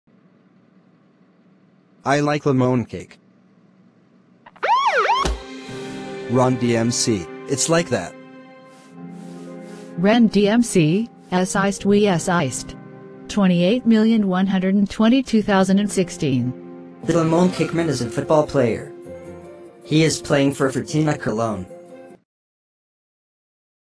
Streit auf dem Sofa | Filme
Was ist denn hier los? Nach diesem Video hat der Lemoncakeman kühlen Orangensaft in ein Glas eingeschüttet und es dann anschließend getrunken.

Programm: Plotagon